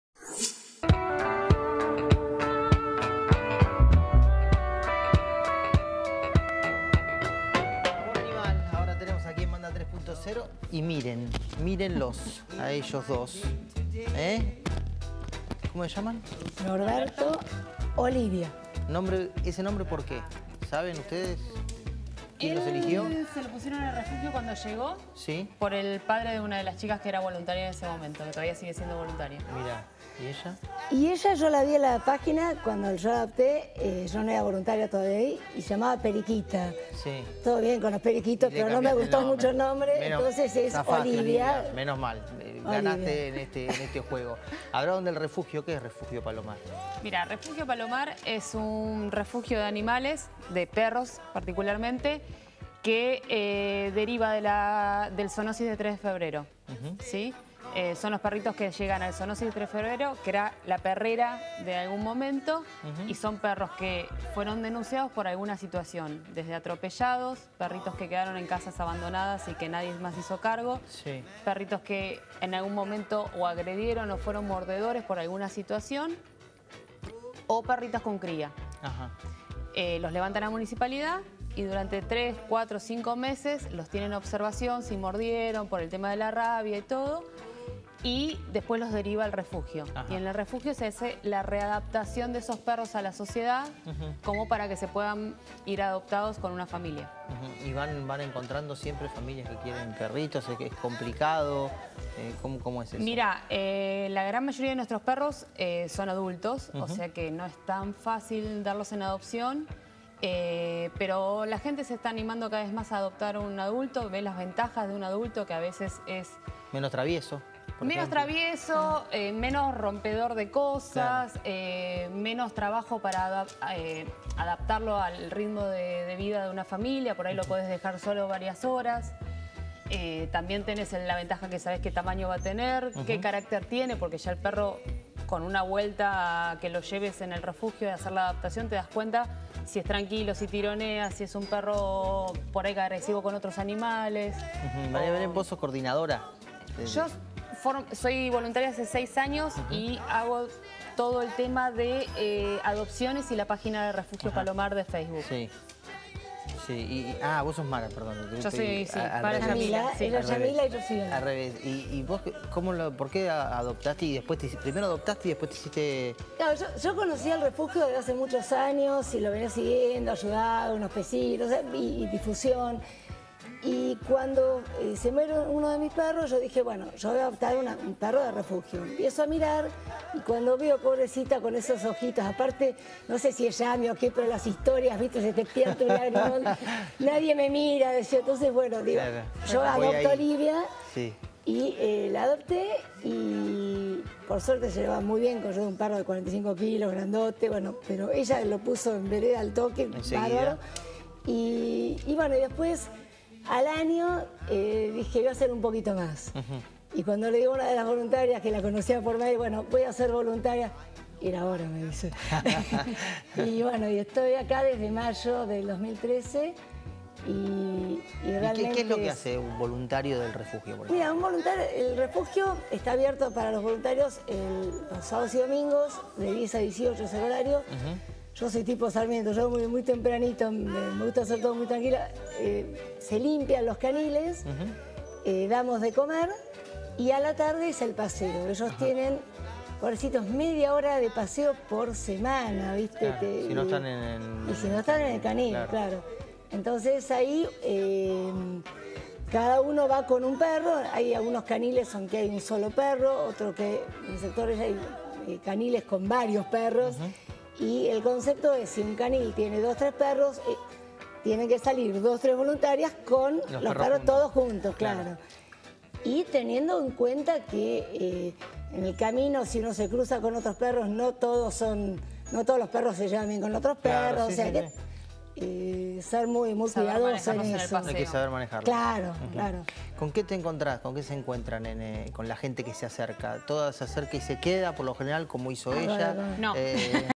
Amor Animal en Canal METRO - 26/11
Amor Animal estuvo en Canal METRO junto a Belén, Mara y Norma del Refugio Palomar.